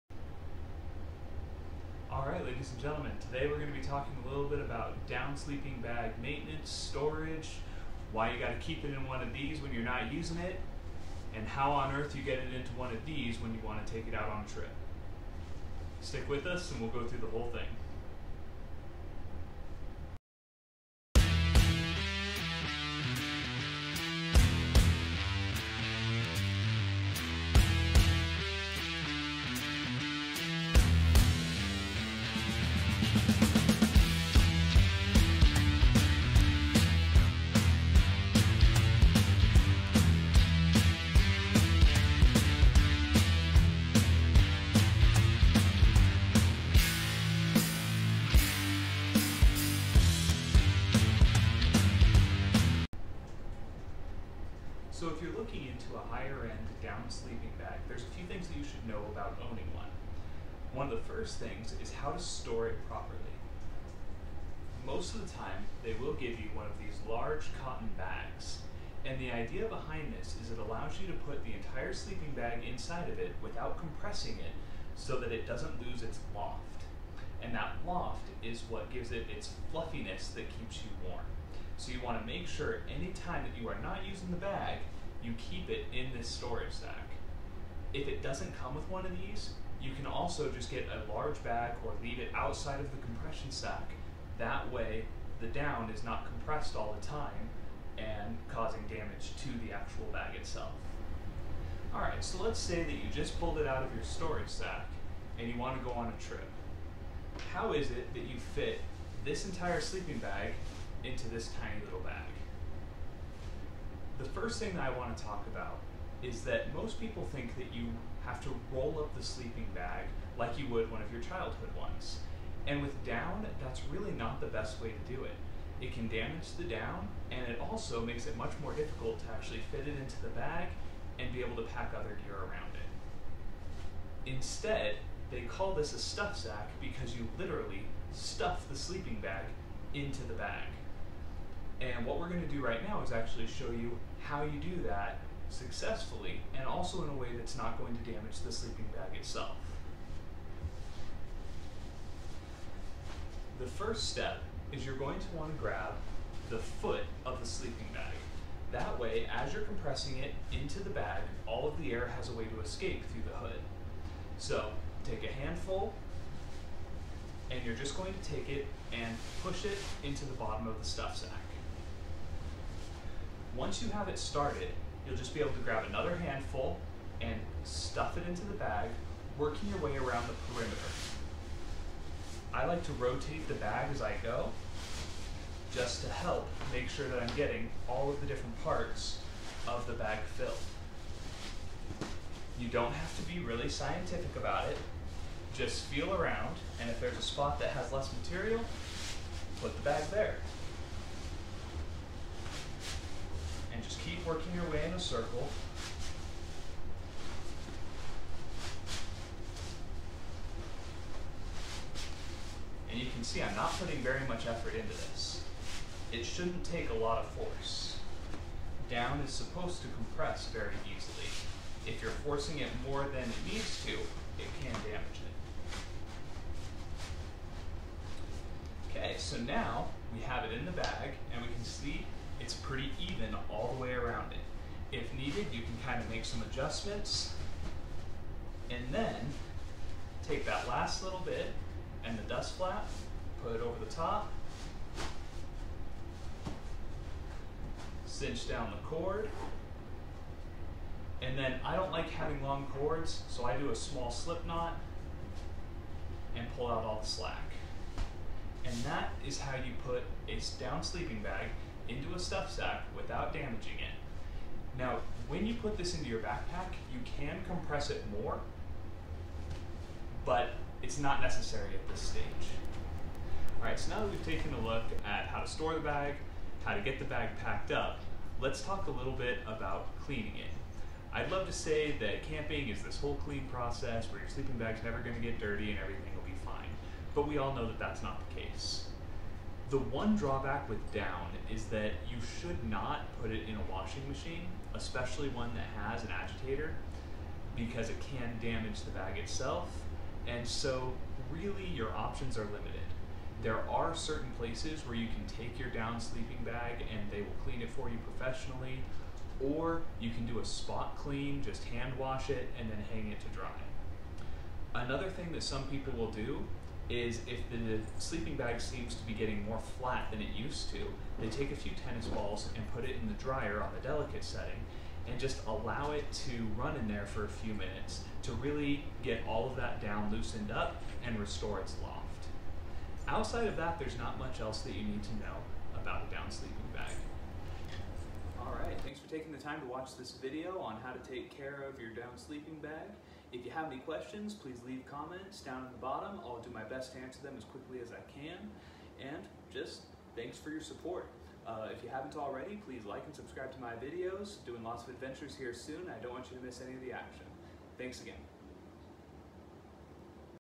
How To Pack, Store, and Clean a Down Sleeping Bag
Down sleeping bags are one of the best pieces of equipment a backpacker can have. They are warm, compact, and lightweight.

This video answers the most common questions about down sleeping bags:
How do I store it? How do I fit it into the stuff sack? How do I clean it? Can I restore its loft?

Please comment if this video did not answer your questions and I will answer them quickly.